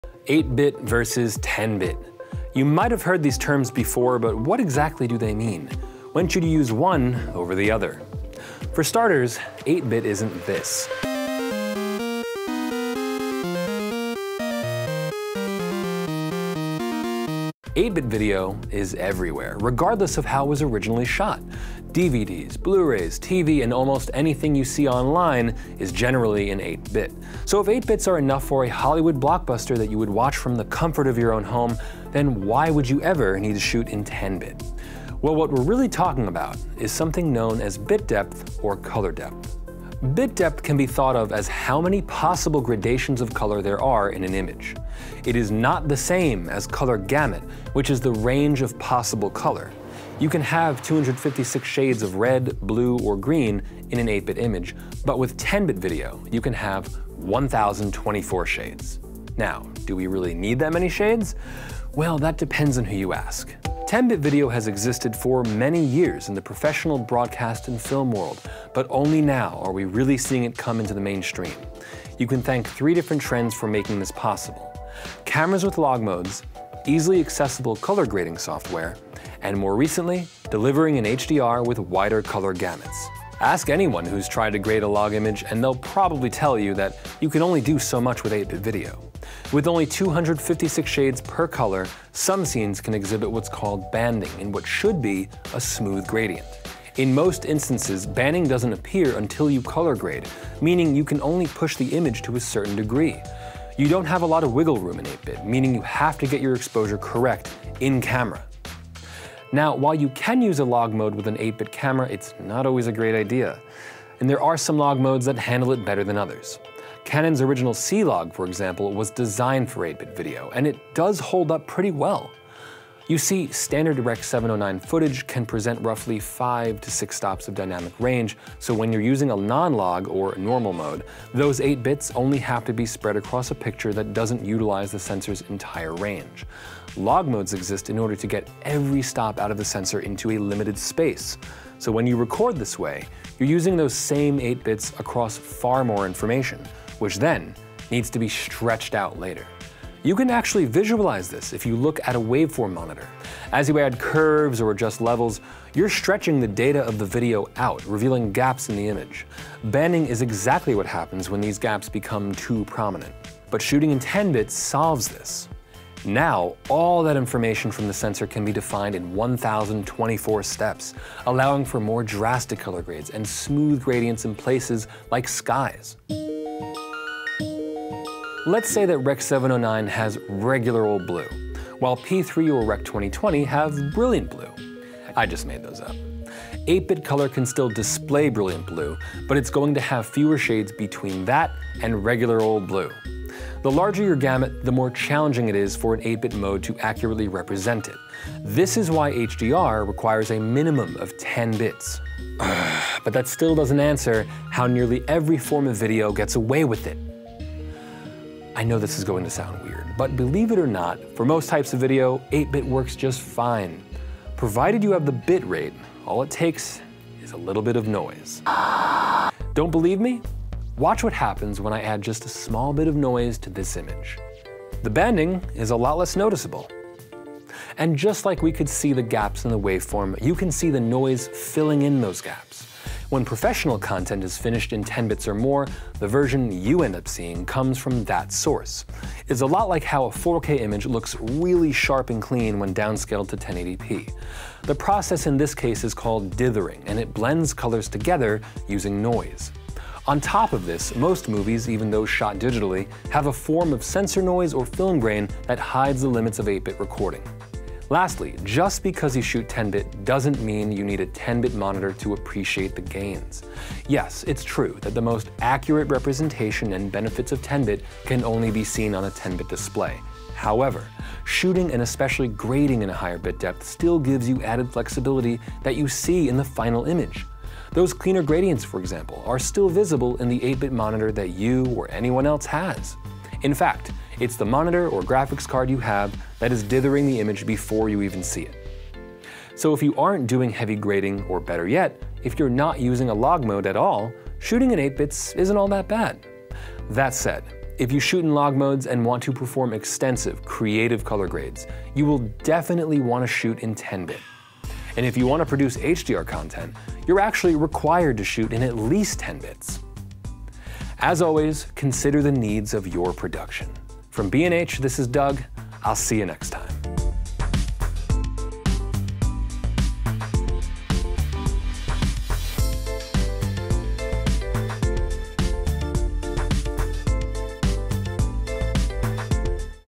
8-bit vs. 10-bit Video | What's the Difference?
What is 10-bit video? Should I shoot it over 8-bit video? In this video, Doug explores what's at the heart of this debate as he explains what bit depth is, and why it matters both in production, and in post.